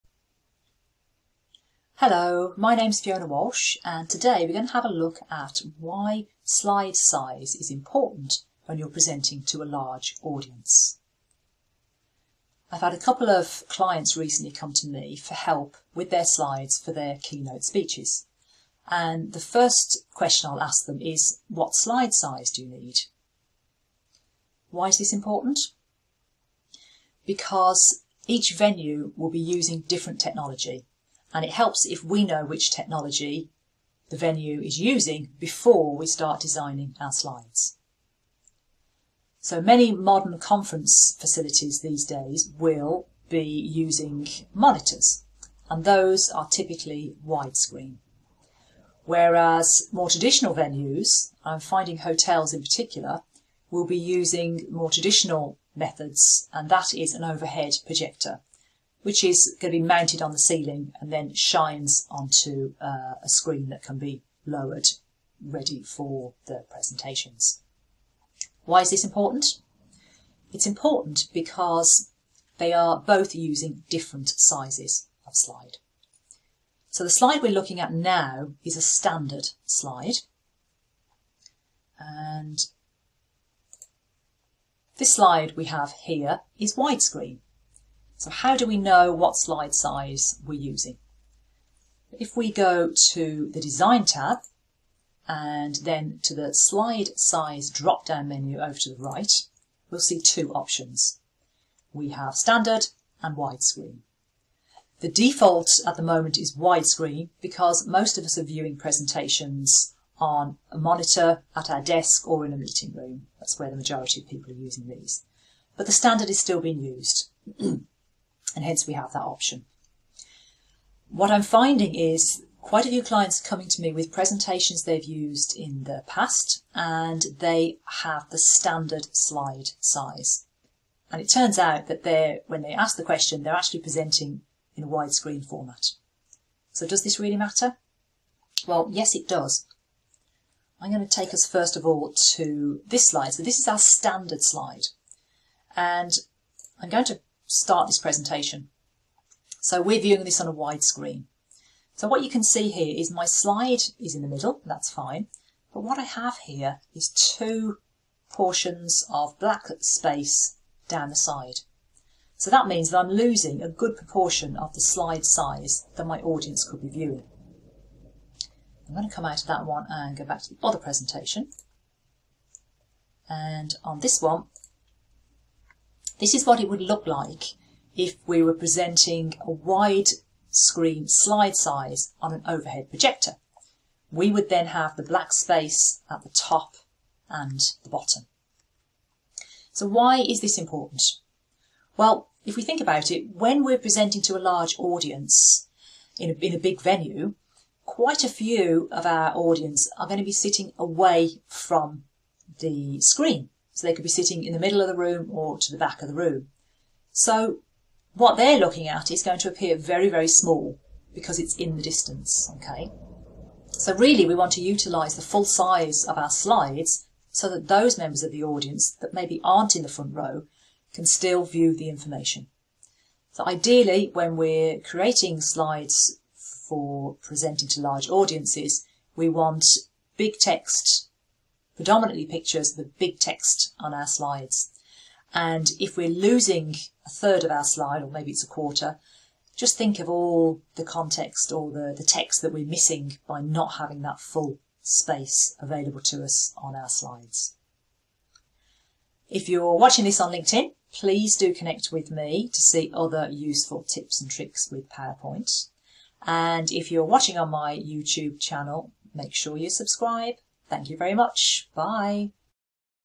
PowerPoint - Get the Right Slide Size for Your Speaking Gig
In this video, I explain the importance of slide size when presenting to a large audience. I also show you how to get the right slide size for any speaking event you may have to prepare a PowerPoint presentation for.

This is important because different venues may use different types of technology. It helps to know which technology you will present your slides on before starting the design process.